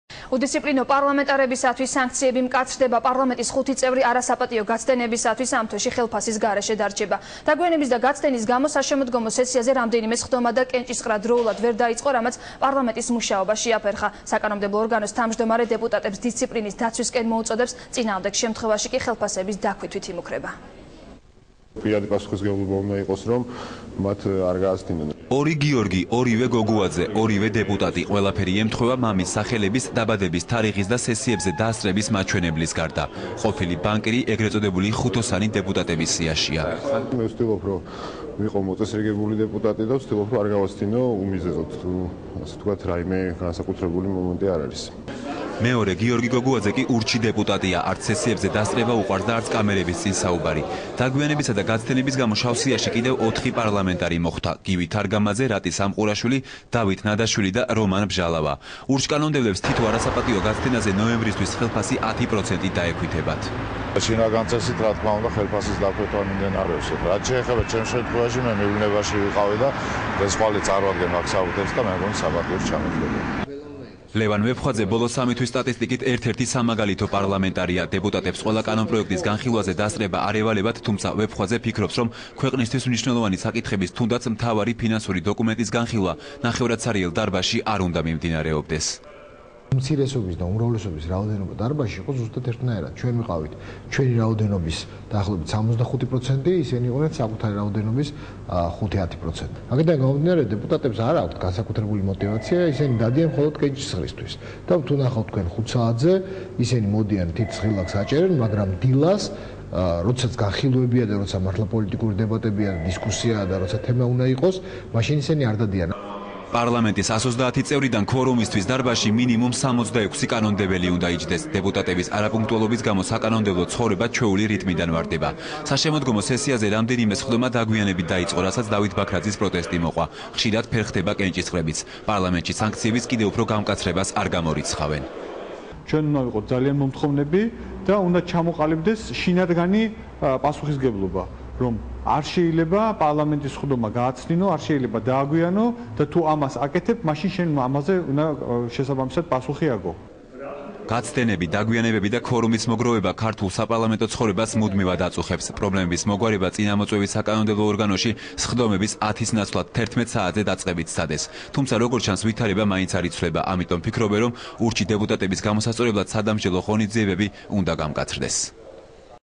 26.11.2009
ურჩ კანონმდებლებს თითო არასაპატიო გაცდენაზე  ხელფასი 10 პროცენტით დაექვითებათ